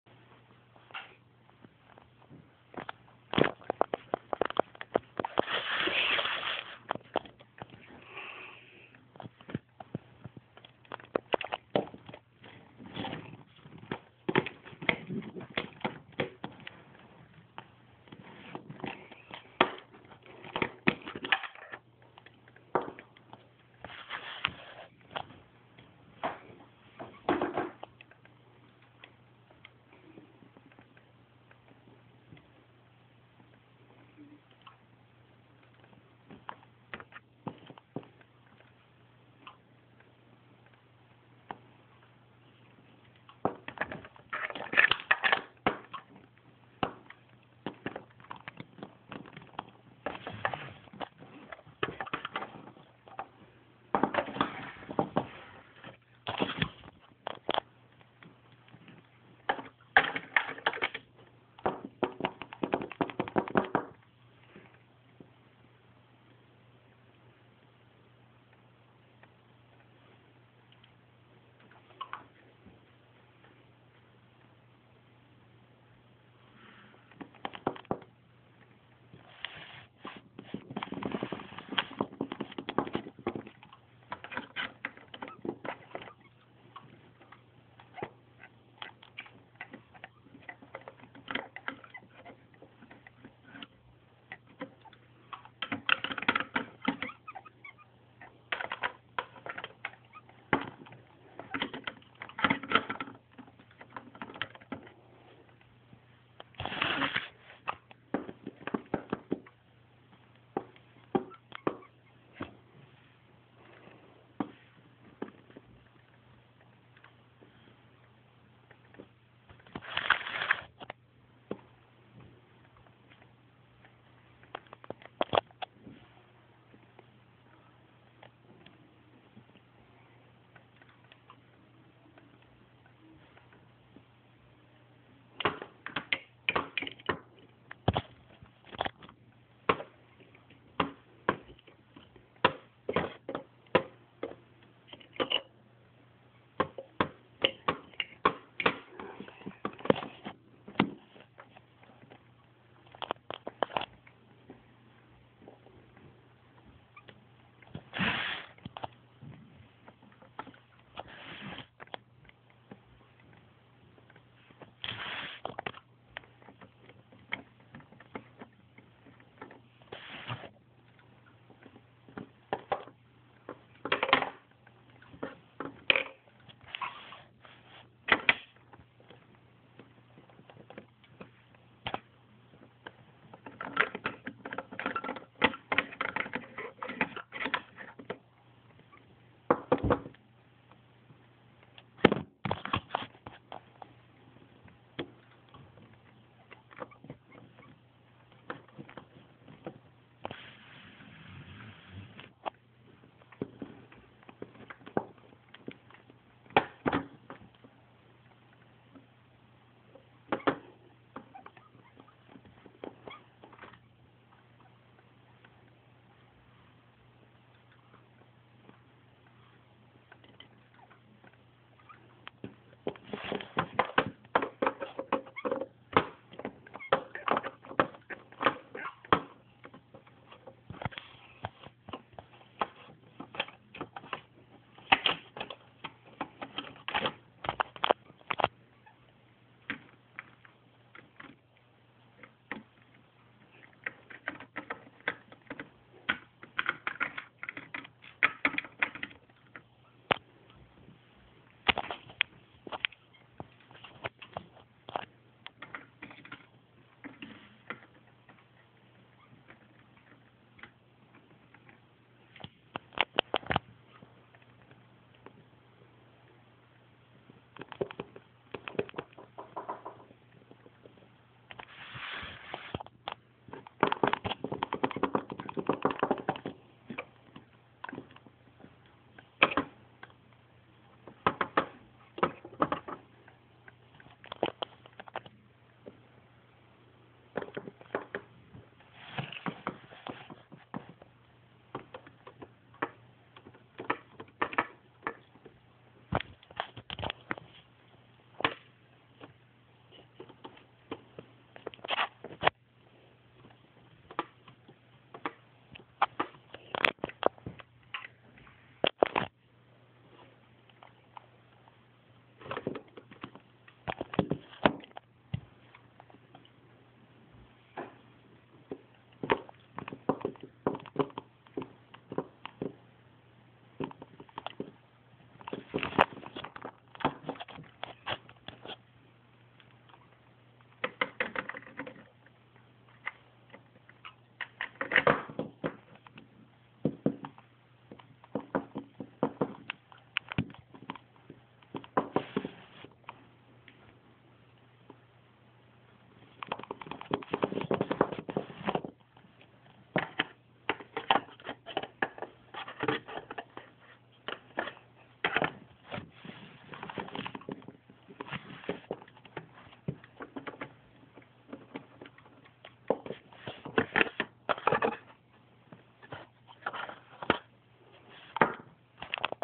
My LPS pets go to the club
The cat and the squirrel go to the club